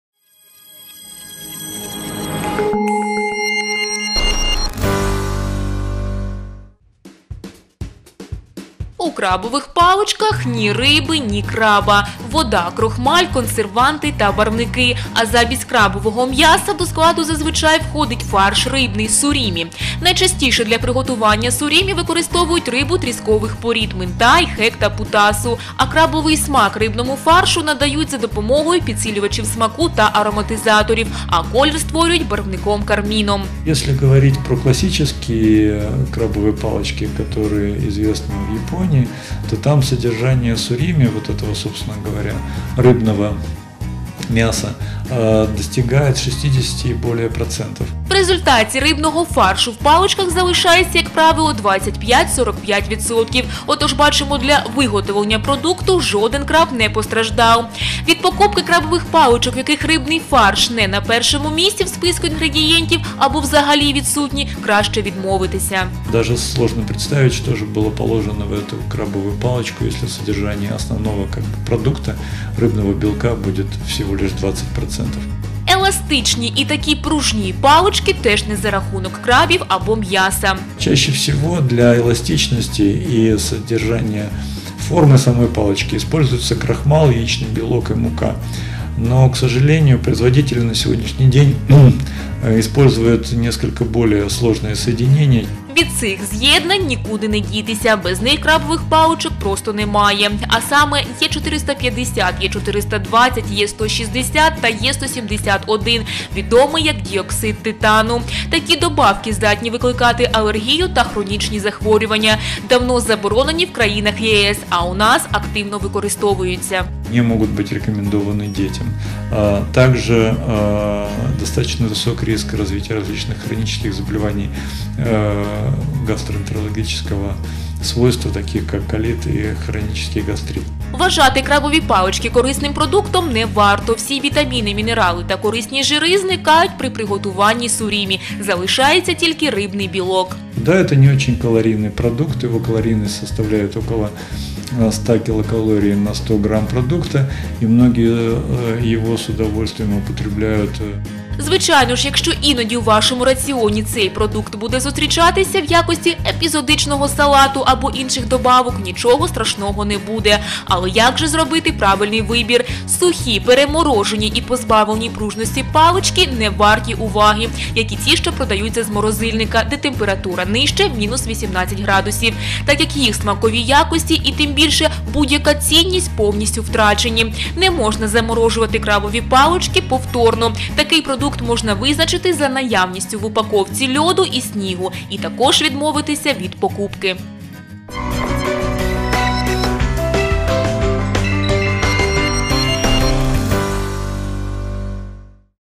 Крабові палички... Урок правильного харчування в рубриці “Їстівна азбука”
Усе про крабові палички знає лікар-дієтолог Роман Мазо.